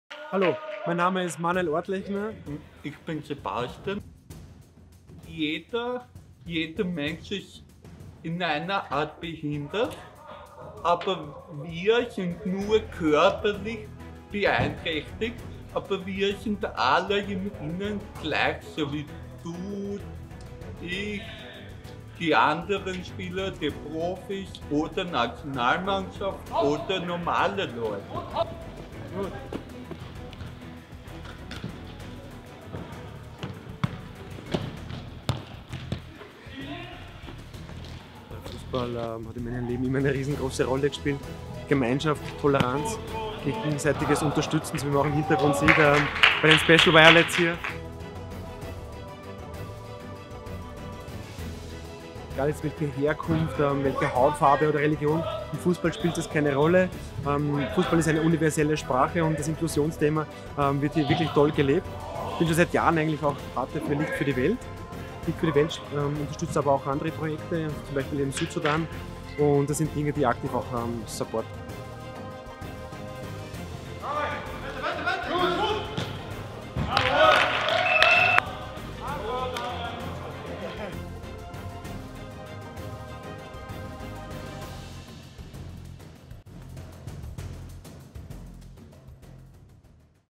Ein besonderes Training mit den FK Austria Special Violets
Zum Internationalen Tag des Sports für Entwicklung und Frieden waren wir gemeinsam mit Ex-Nationalspieler und Licht für die Welt-Kinderpate Manuel Ortlechner bei einem Training der FK Special Violets dabei. Das ist die inklusive Kraft von Sport.